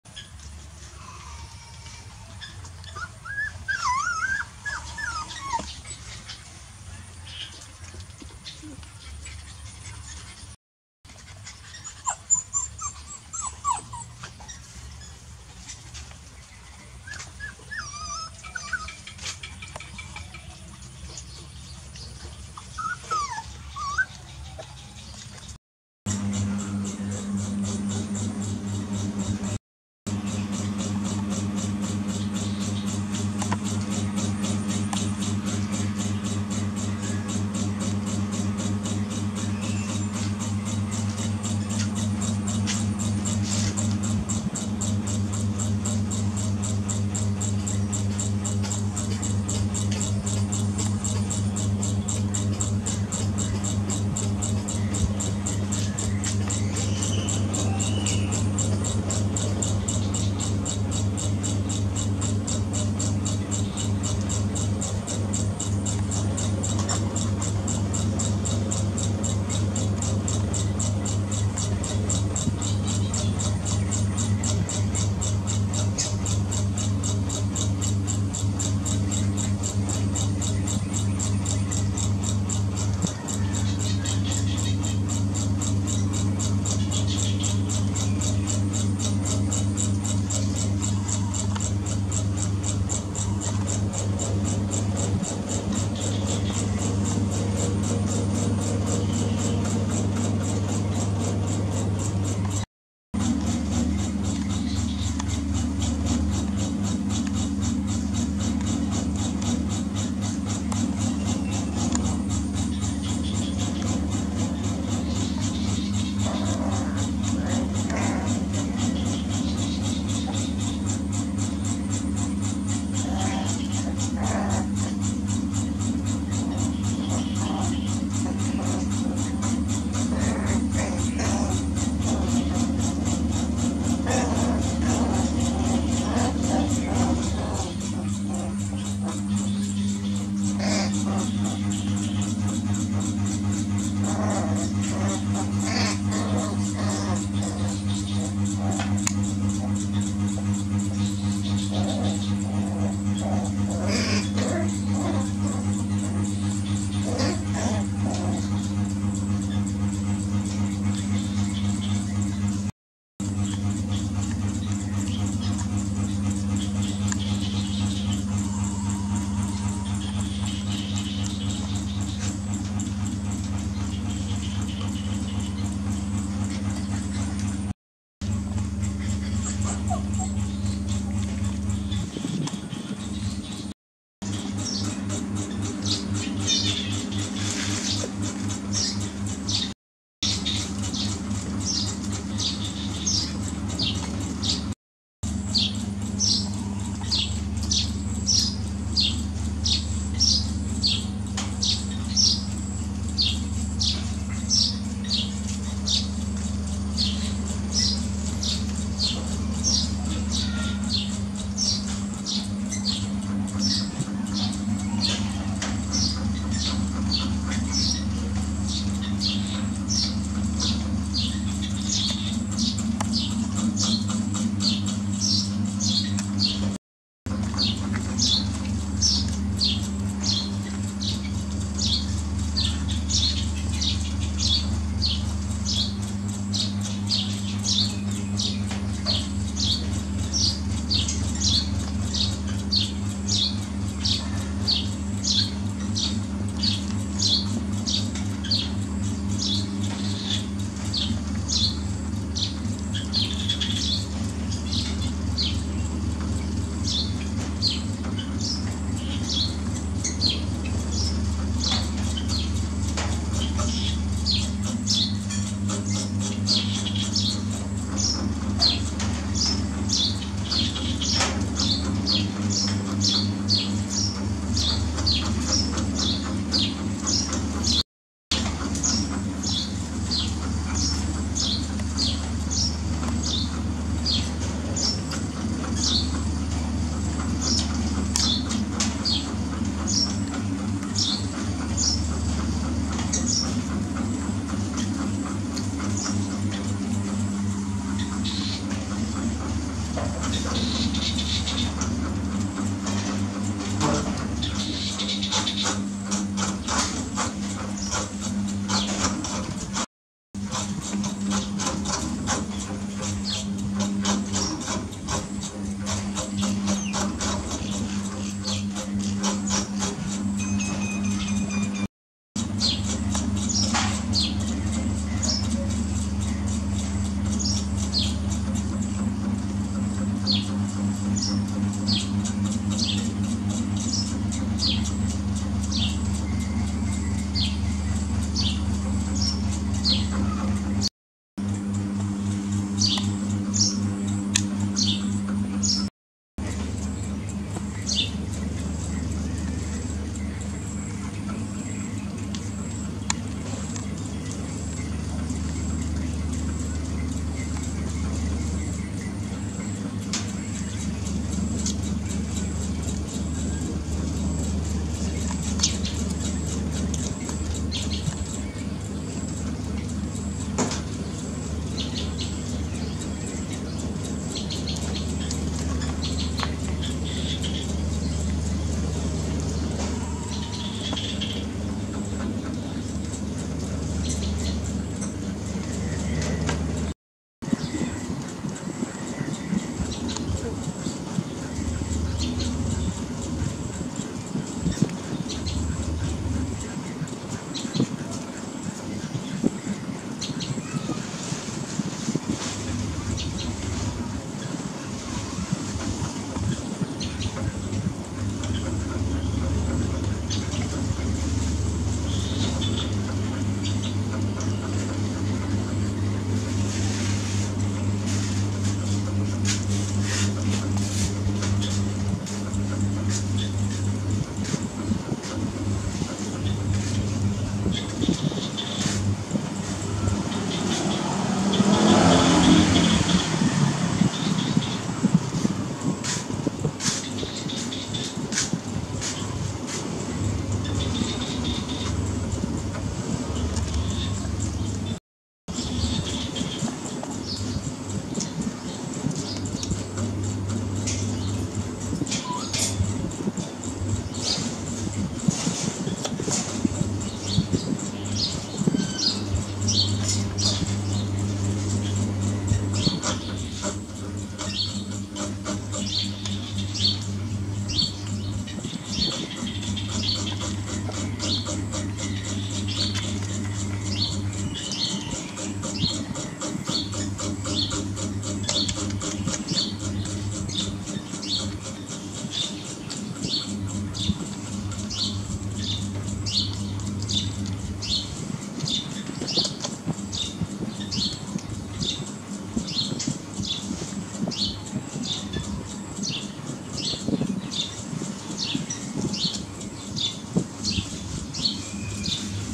OMG very sweet milk baby ToTo drink many milk he is very fat and good heal
Best wishes
Crews Domestic OMG very sweet milk baby ToTo drink many milk he is very fat and good heal